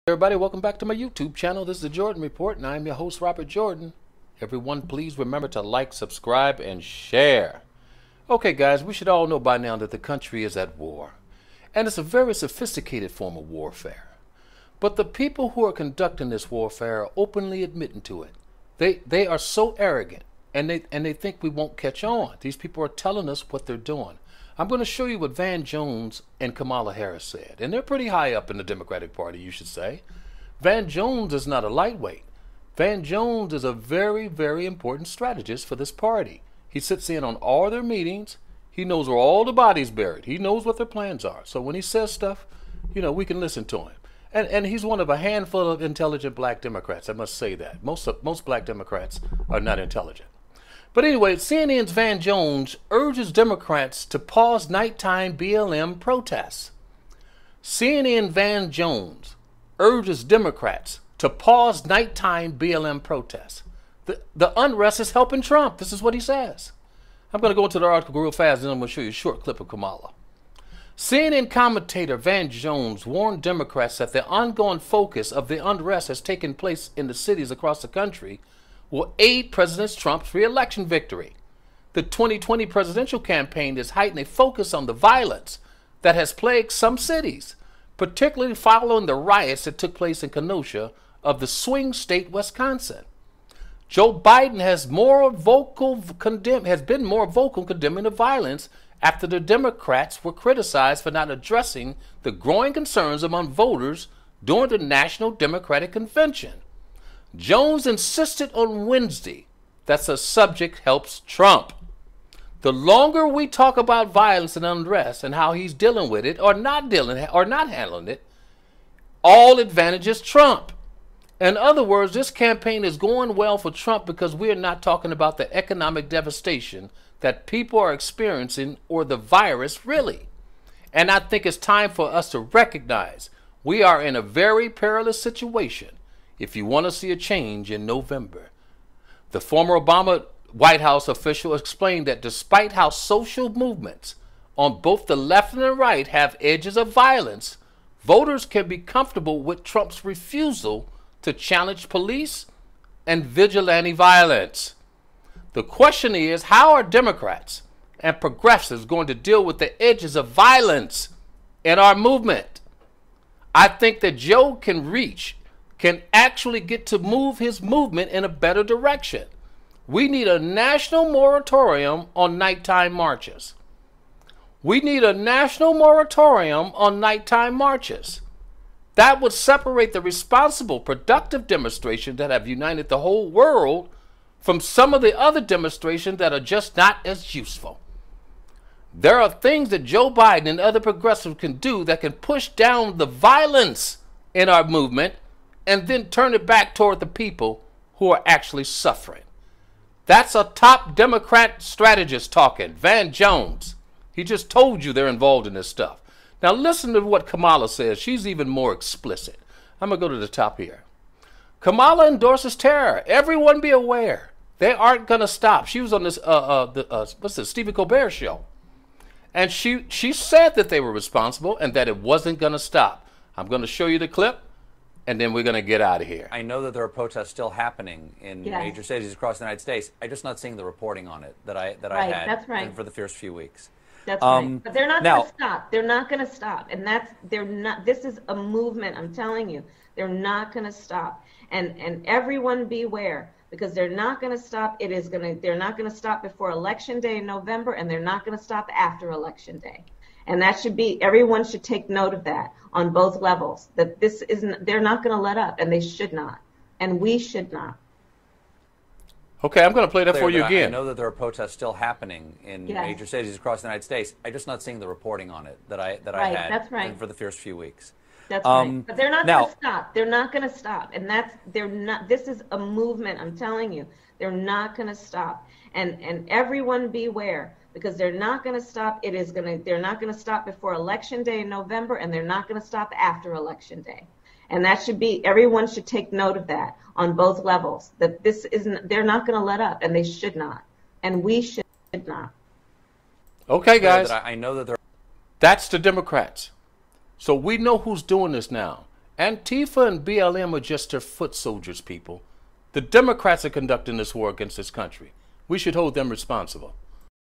Kamala Harris says Beware the Riots 'aren't gonna stop'!
Kamala Harris warned or threaten us that the violence we're seeing isn't going to stop before or after the election!